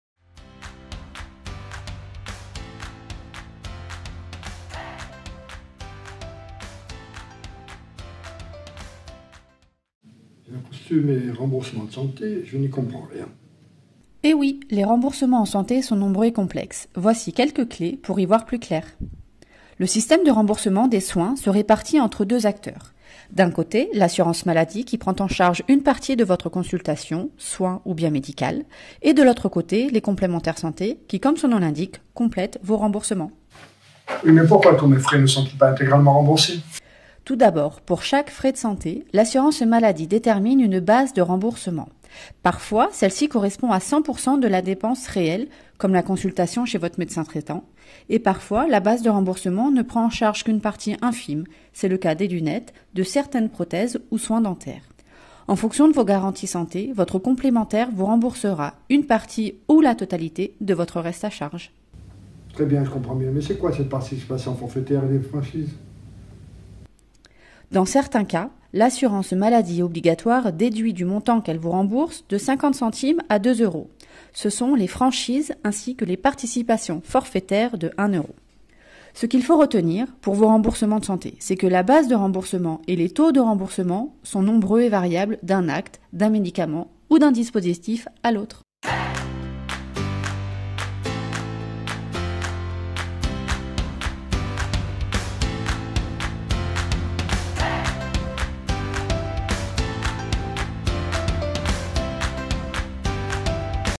Episode 1 : Comprendre mes remboursements
Le système de remboursement des soins repose sur une intervention combinée de l’Assurance maladie obligatoire et des organismes complémentaires santé. Cependant il n'est pas toujoujrs facile d'en comprendre le fonctionnement. Voici quelques clés pour y voir plus clair !